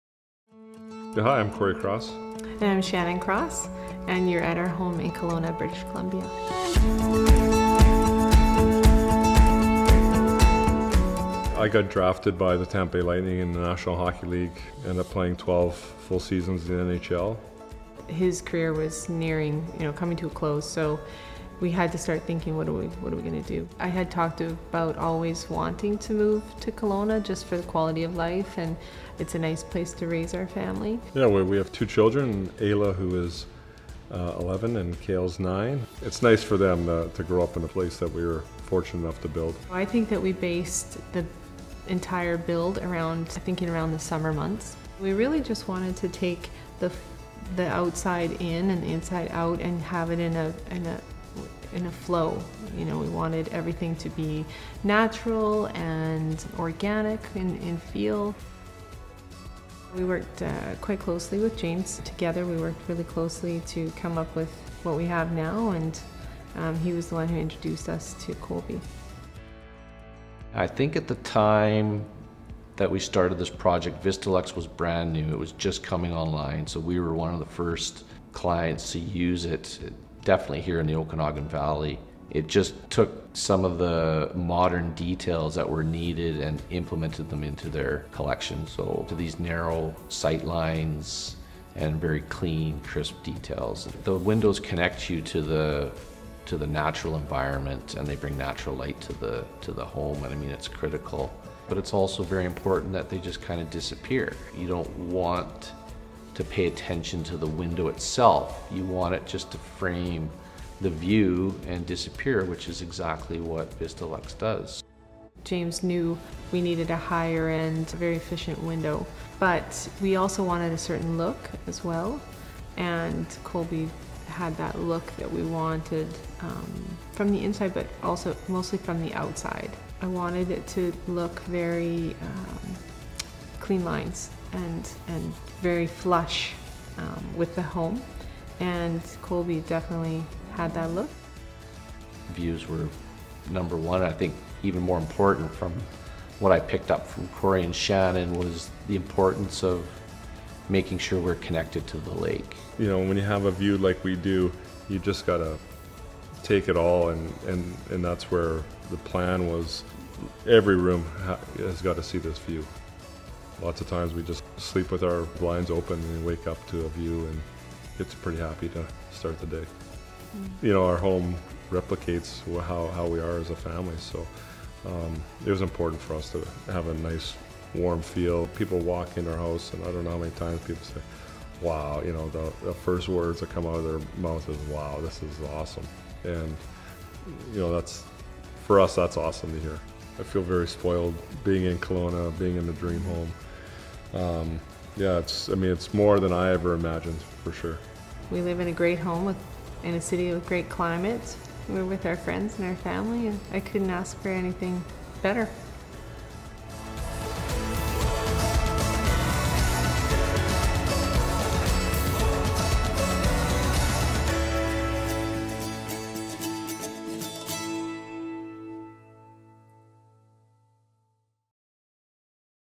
Overlooking Okanagan
Following his professional hockey career, Cory and Shannon Cross build a lakeside residence on a pine tree-covered hill that captures views of Canada’s shimmering Okanagan Lake.  The couple chose Kolbe's VistaLuxe® Collection windows and doors to frame their incredible lake views. (Modern Views: Kelowna, BC)